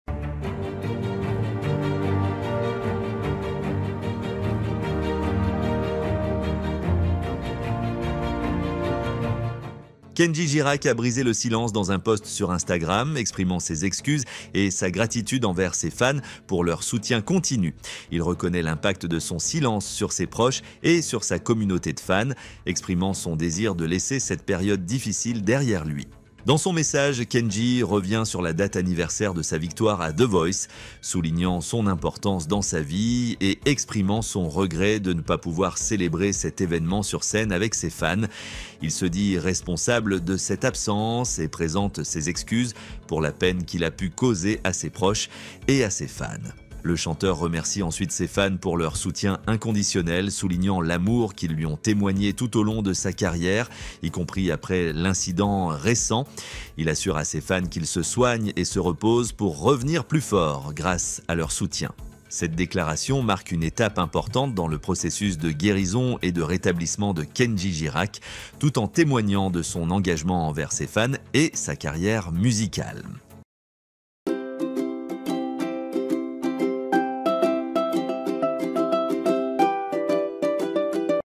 Kendji Girac s'exprime enfin Son premier témoignage sur l'incident dramatique
Kendji Girac a brisé le silence dans un post sur Instagram, exprimant ses excuses et sa gratitude envers ses fans pour leur soutien continu. Il reconnaît l'impact de son silence sur ses proches et sur sa communauté de fans, exprimant son désir de laisser cette période difficile derrière lui.